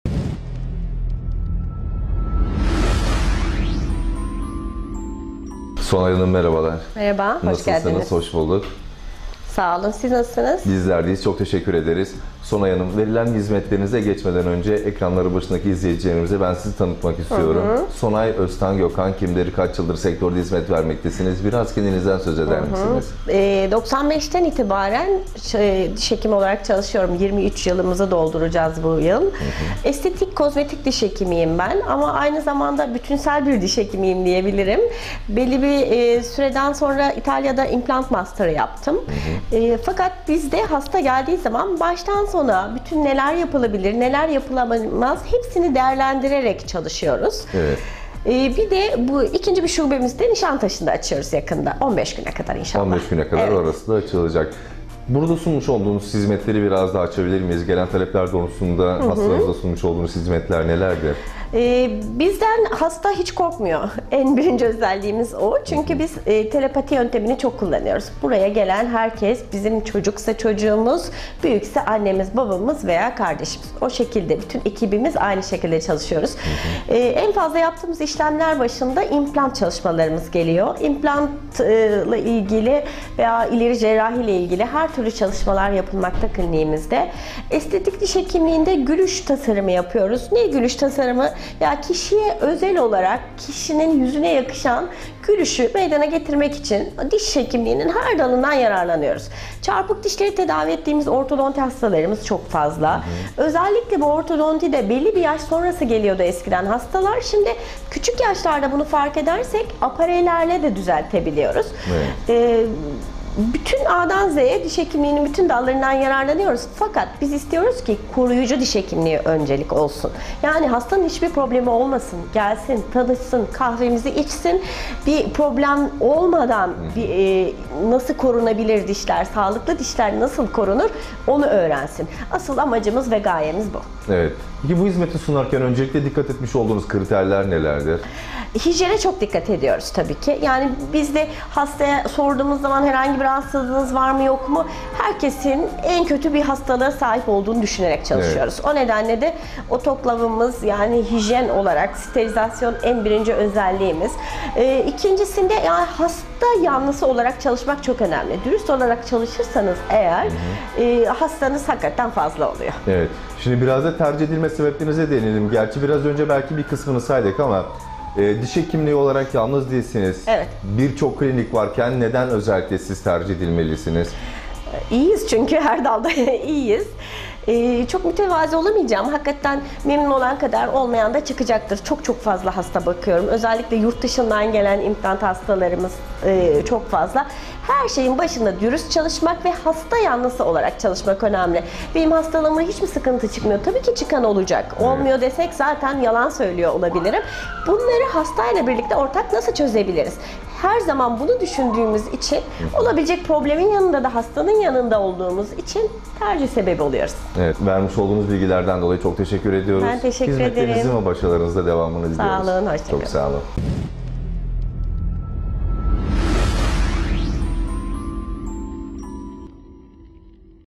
ALANINDA UZMAN DİŞ HEKİMİ I ESTETİK DİŞ HEKİMİ SONAY ÖZTAN
1995’ten beri diş hekimi olarak çalışan Sonay Öztan Gökhan, verdiği hizmetleri Posta Kobi'ye anlattı.